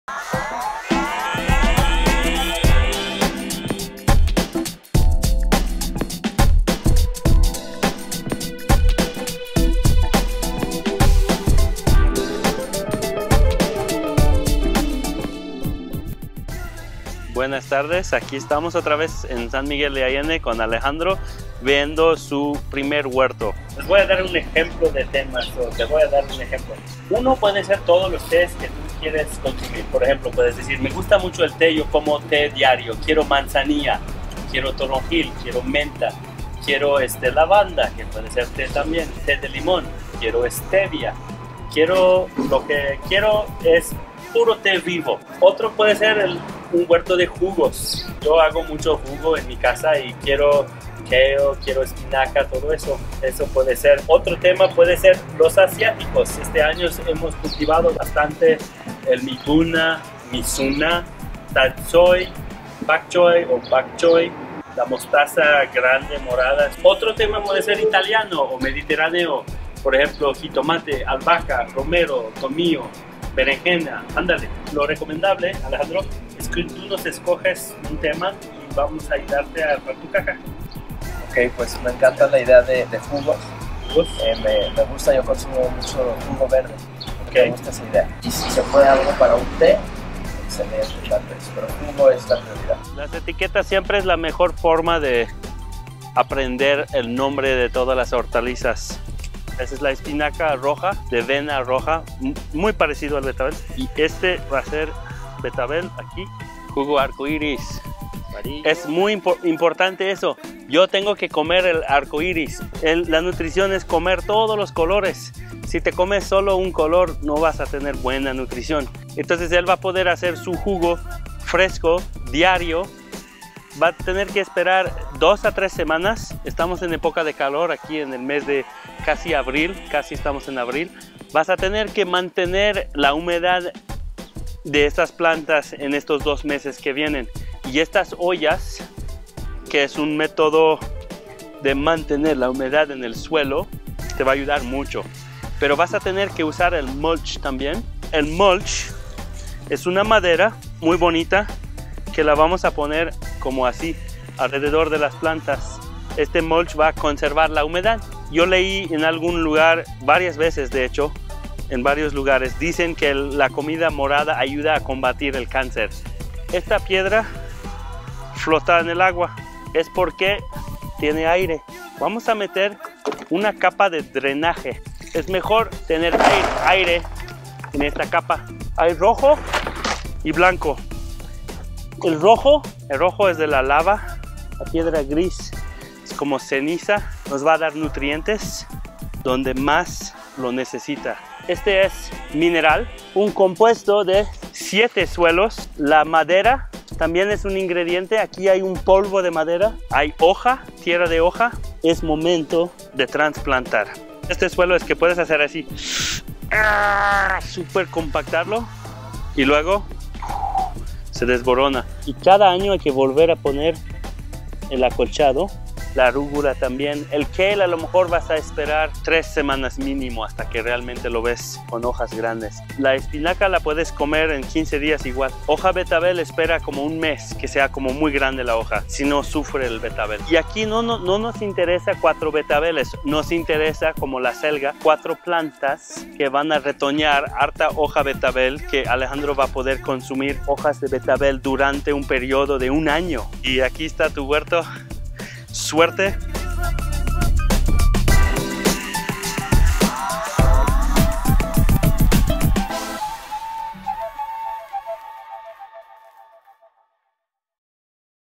Bodega Orgánica - Taller de Huertos Urbanos con Alejandro Trejo
En lugar de reciclar damos un mejor uso a cajas bachoco - los convertimos en jardineras para azoteas y patios para crear huertos urbanos de bajo costo pero gran impacto.